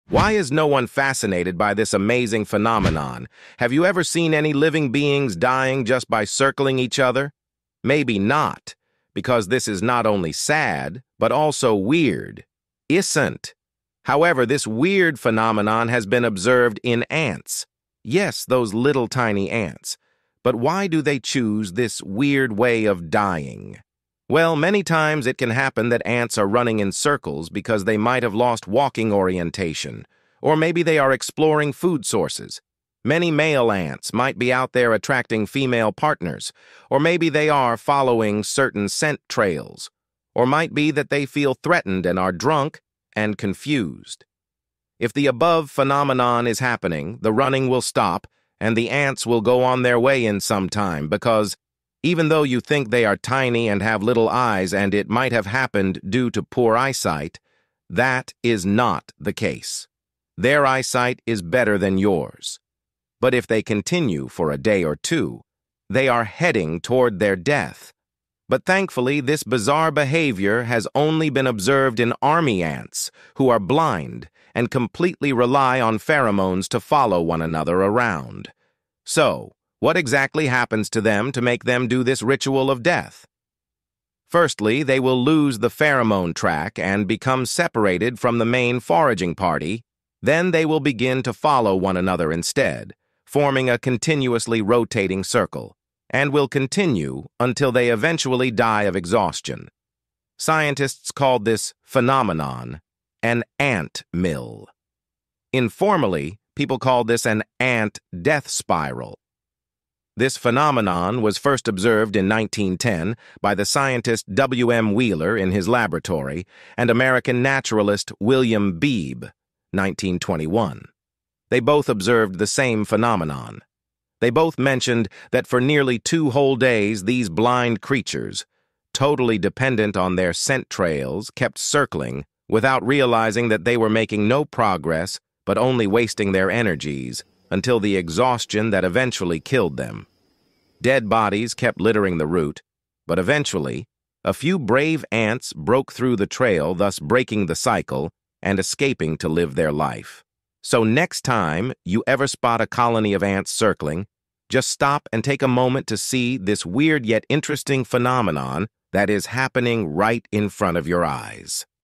what happens when ants run in circles
Have you ever seen any living beings dying just by circling each other? Maybe Not, because This is not only Sad but also weird. Isnt?
However, this weird phenomenon has been observed in Ants. Yes, those little tiny ants. Watch Until the end to know what happens when ants run in circles?